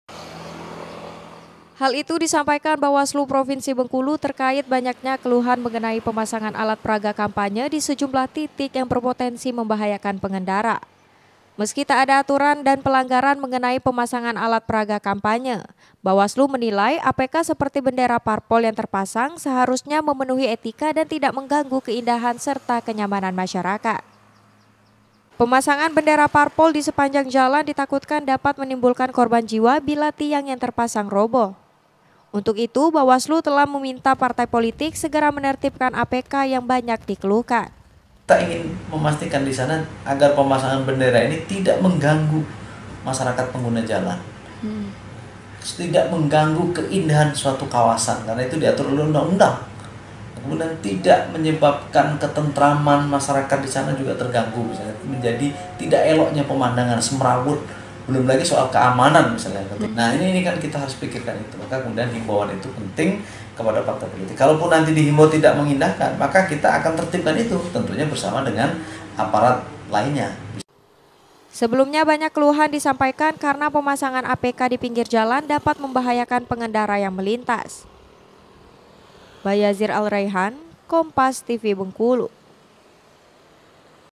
Bawaslu Minta Parpol Copot APK Yang Semrawut
BENGKULU, KOMPAS.TV – Hal itu disampaikan Bawaslu Provinsi Bengkulu terkait banyaknya keluhan mengenai pemasangan alat peraga kampanye di sejumlah titik yang berpotensi membahayakan pengendara.

Meski tak ada aturan dan pelanggaran mengenai pemasangan alat peraga kampanye, Bawaslu menilai APK seperti bendera parpol yang terpasang seharusnya memenuhi etika dan tidak mengganggu keindahan serta kenyamanan masyarakat.

Pemasangan bendera parpol di sepanjang jalan ditakutkan dapat menimbulkan korban jiwa bila tiang yang terpasang roboh.

Untuk itu Bawaslu telah meminta partai politik segera menertibkan APK yang banyak dikeluhkan.

Sebelumnya banyak keluhan disampaikan karena pemasangan APK di pinggir jalan dapat membahayakan pengendara yang melintas.